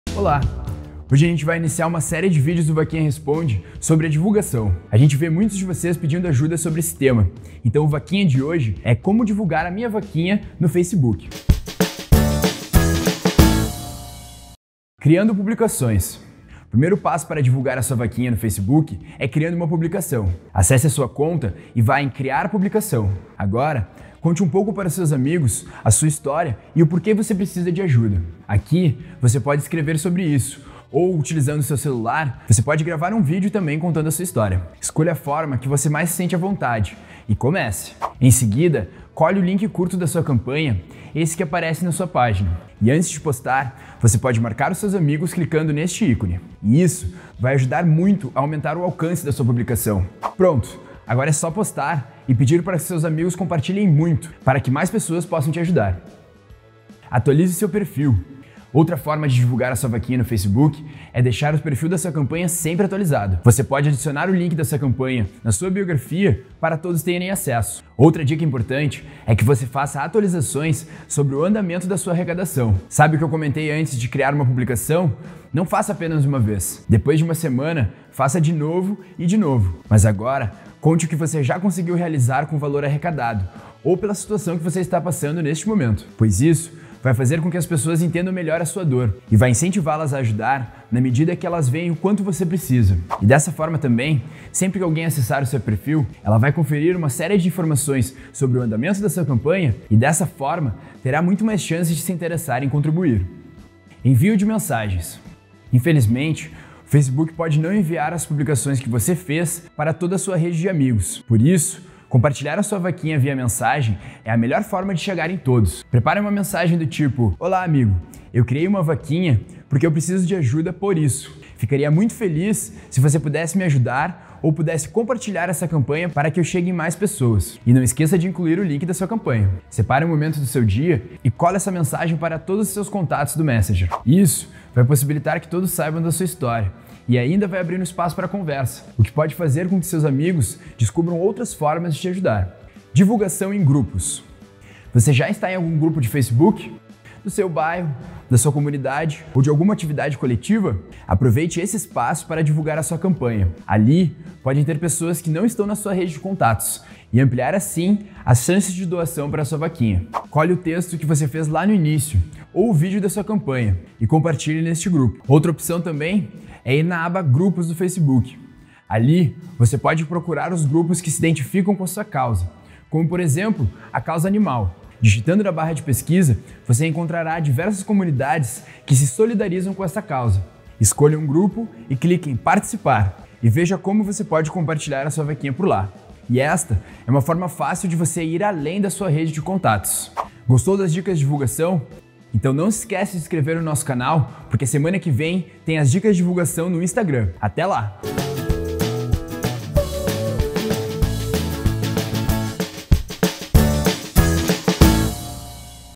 VakinhaResponde - Como Divulgar Vaquinha no Facebook?
A divulgação é a chave de sucesso para qualquer campanha! Por isso, a equipe do Vakinha preparou uma série de vídeos que vão te ajudar com essa etapa tão importante para você arrecadar doações online.

O primeiro vídeo da série é sobre como divulgar a sua vaquinha no Facebook. Neste material você vai conferir dicas para criar publicações, atualizar seu perfil, enviar mensagens e ainda utilizar grupos como parte da sua estratégia de divulgação.

Se você ainda tem alguma dúvida sobre como utilizar o Vakinha, é só deixar aqui nos comentários!